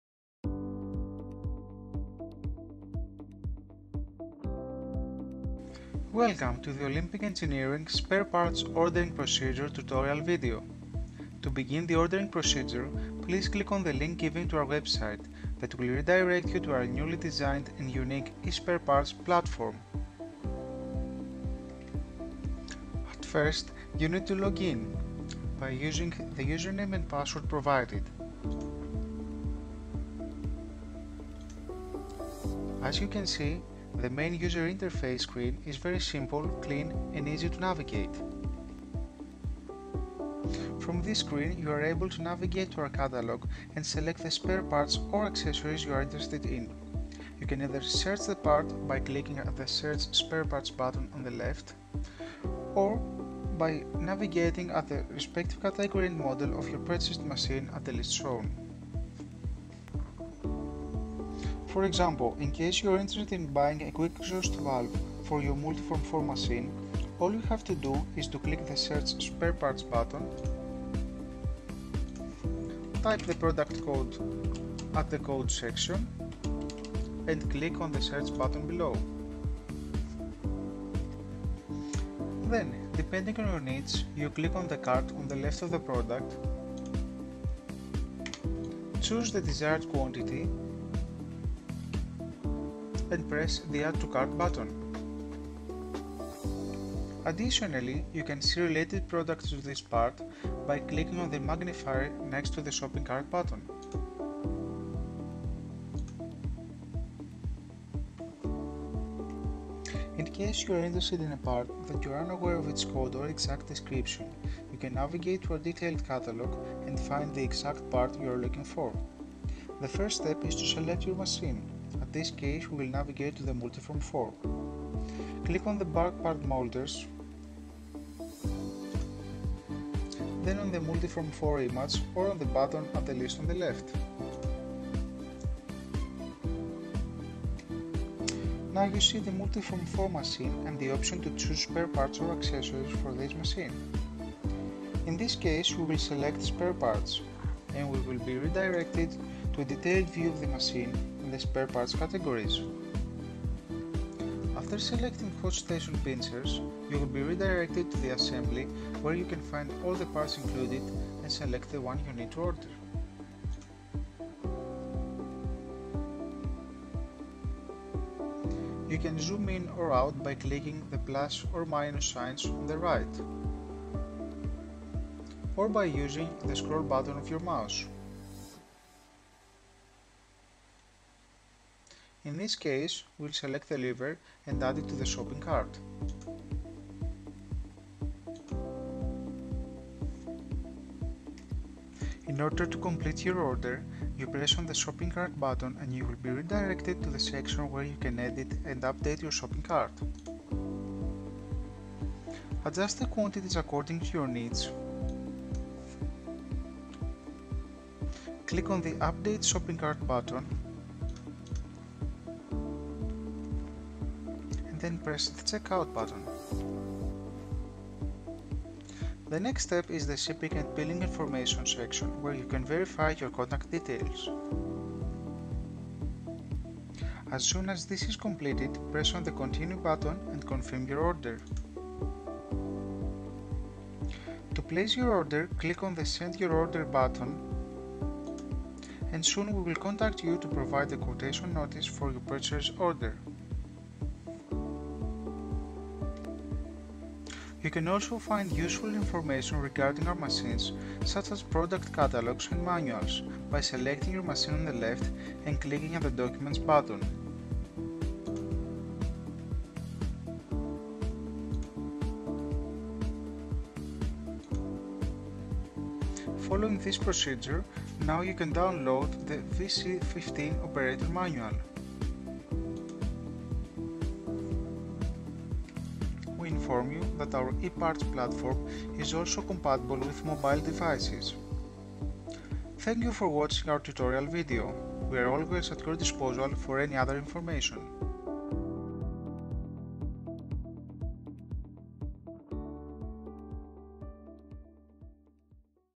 Olympic Engineering - E-parts platform tutorial video
Tutorial video of Olympic engineering e-parts platform. A detailed guide created to assist you in ordering spare parts from our innovative e-parts platform.